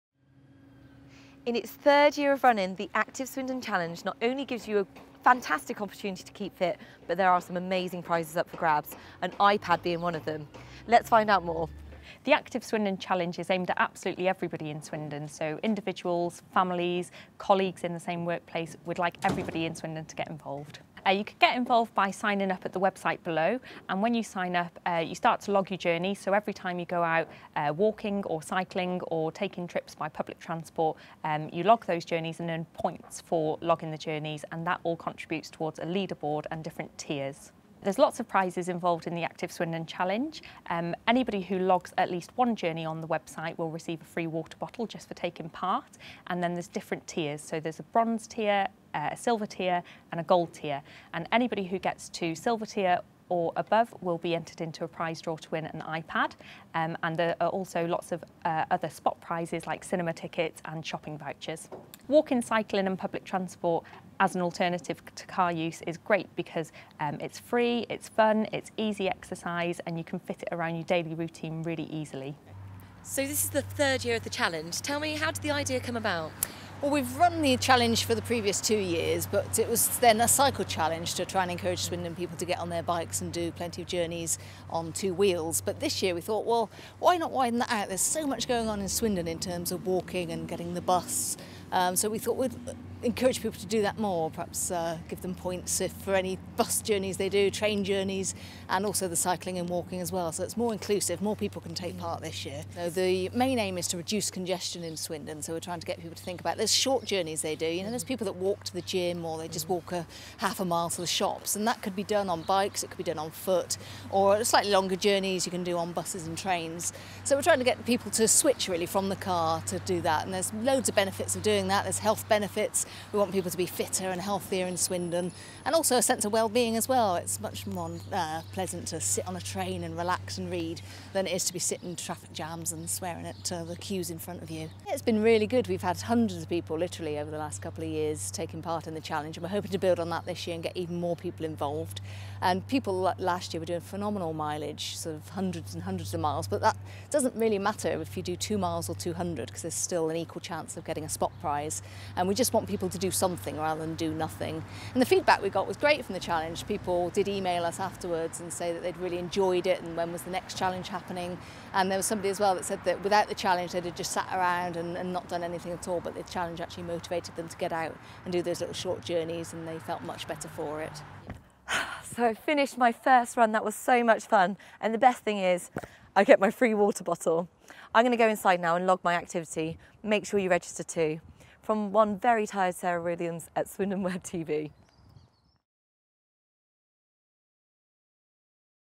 Active Swindon Challenge 2014
We sent Sarah Williams along to find out more about the 2014 Active Swindon Challenge.